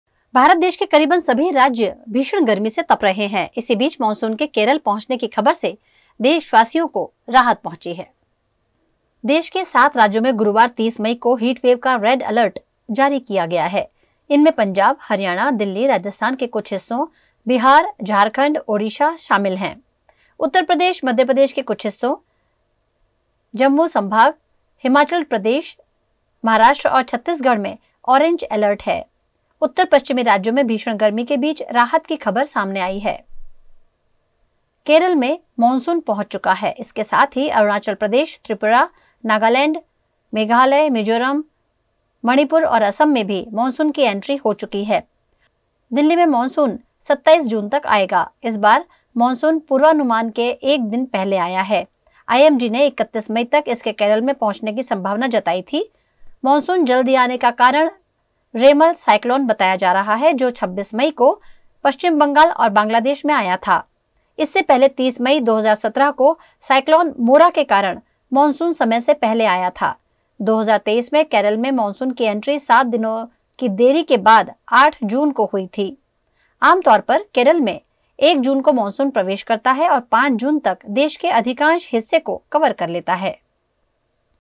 तपती गर्मी के बीच केरल पहुंचा मानसून
भारत देश के करीबन सभी राज्य भीषण गर्मी से तप रहे हैं,इसी बीच मानसून के केरल पहुंचने की खबर से देशवासियों को राहत पहुंची है।